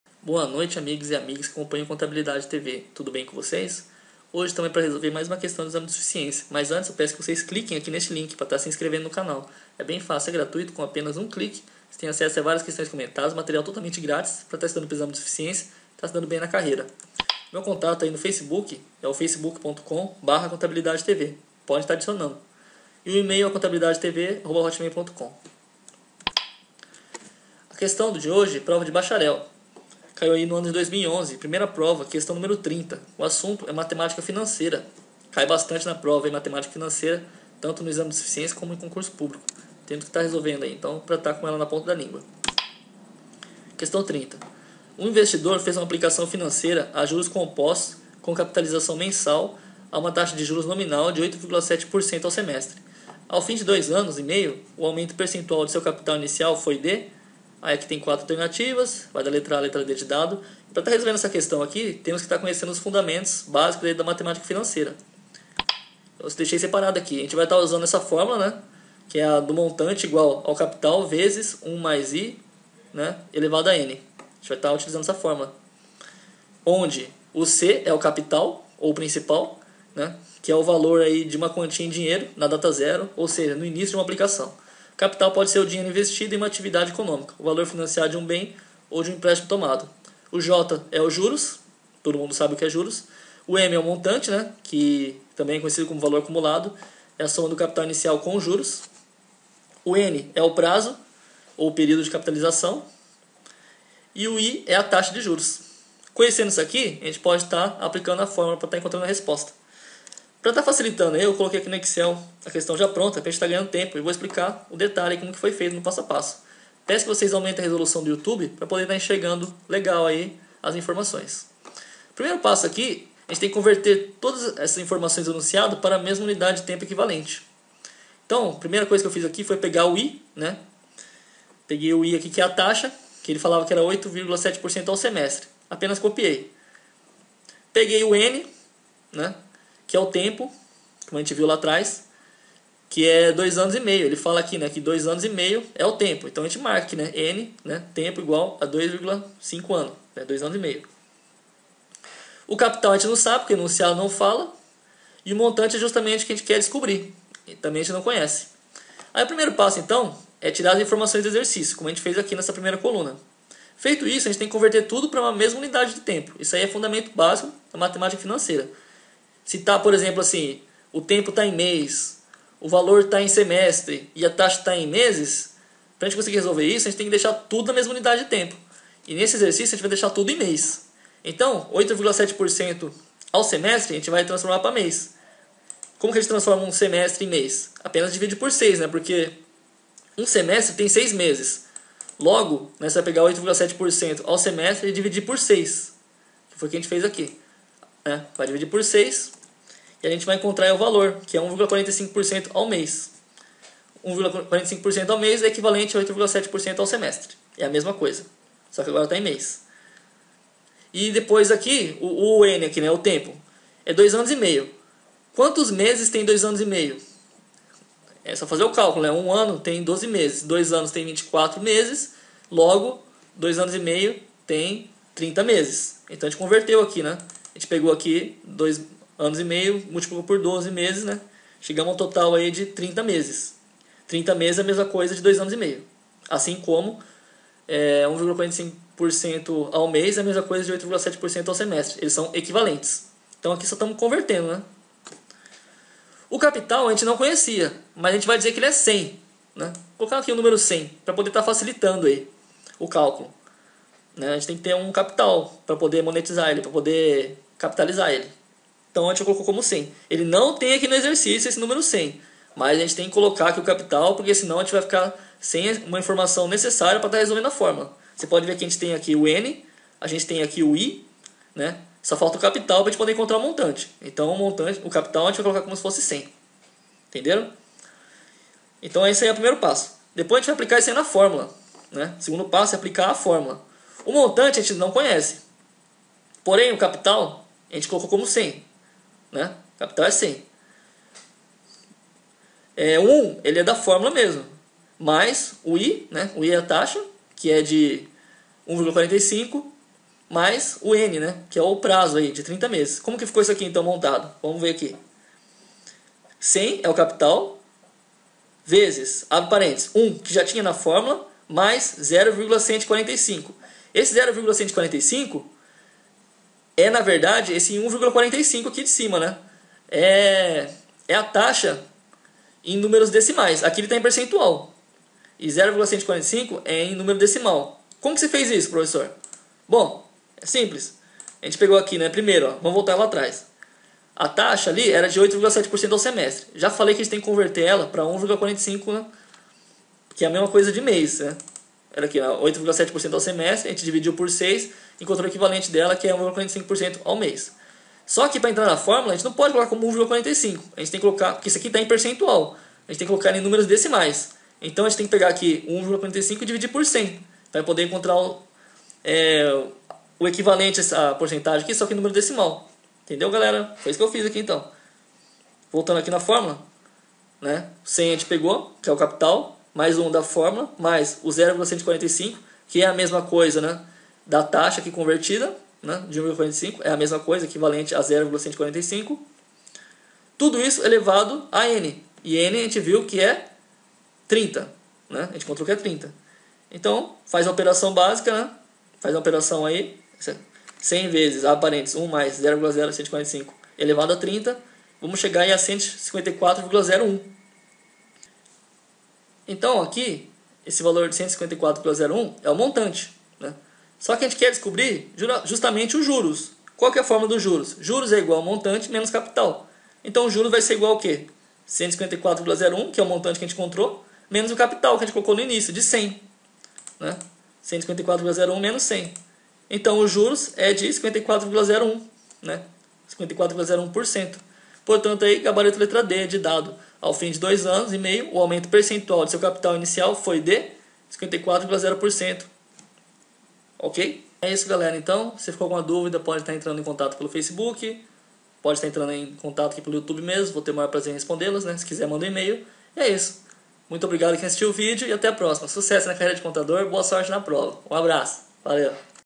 Questão 30 exame de suficiencia 2011 prova 1 - Matemática financeira
30. Um investidor fez uma aplicação financeira a juros compostos com capitalização mensal a uma taxa de juros nominal de 8,7% ao semestre. Ao fim de dois anos e meio, o aumento percentual de seu capital inicial foi de:
a) 43,50%.
b) 49,34%.
c) 51,76%.
d) 54,01%.

GABARITO D